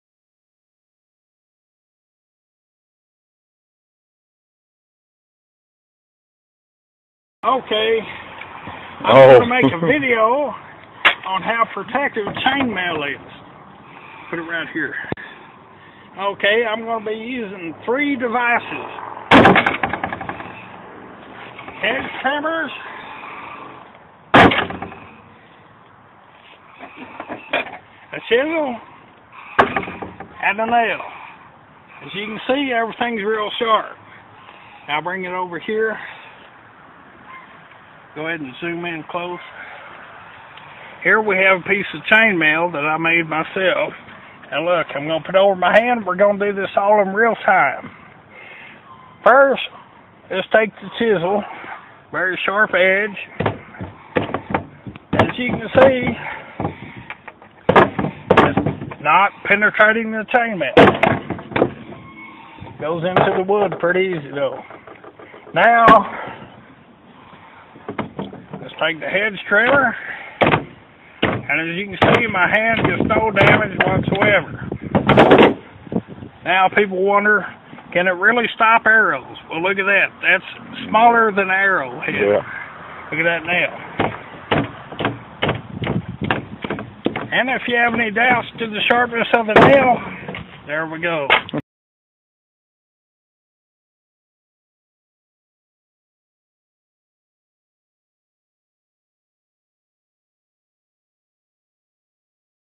CHAINMAILLE ARMOUR DEMO.wmv
A short video showing the effectiveness of maille in protecting the body.